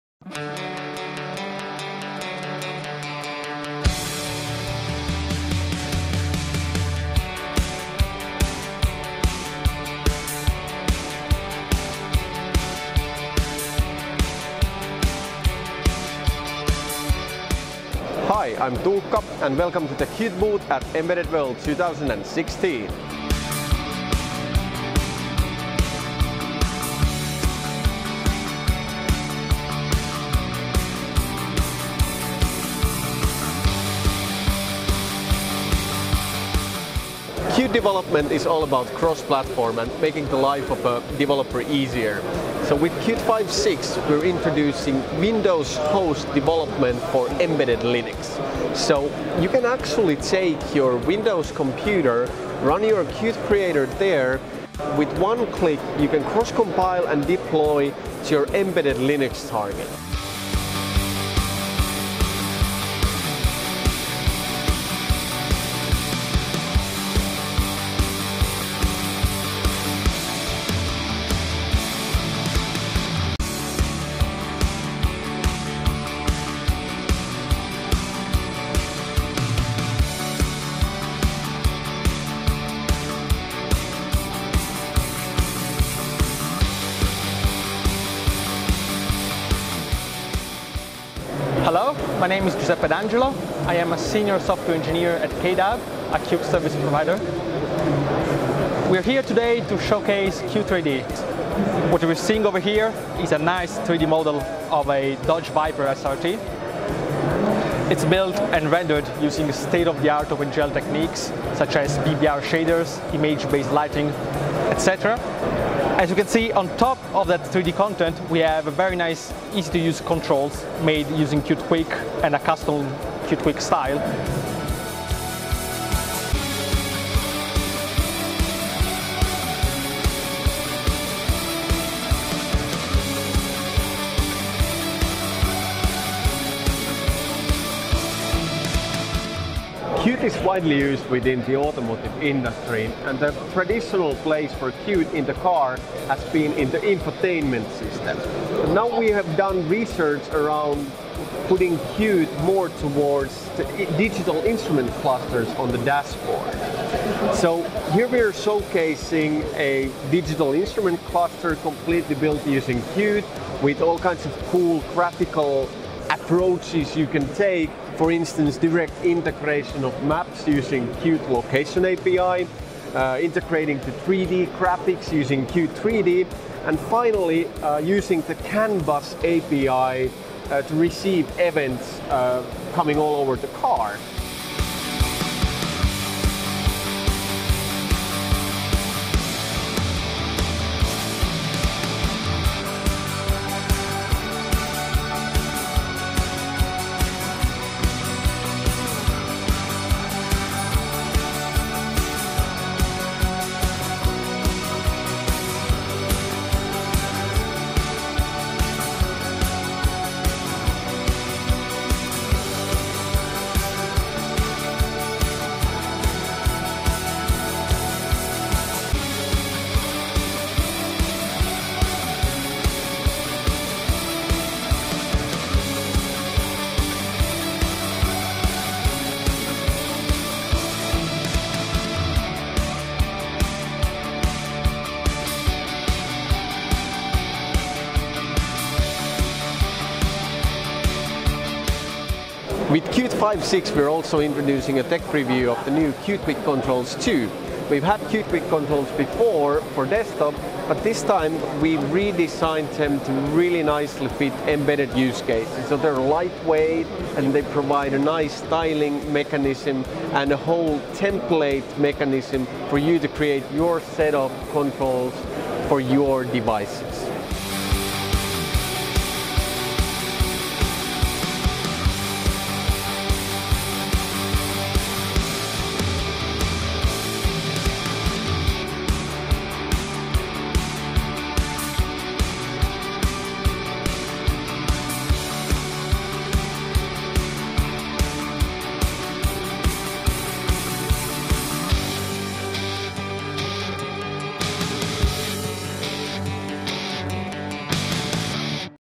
Qt at Embedded World 2016
Feelings and Qt showcases from the Qt booth at Embedded World 2016!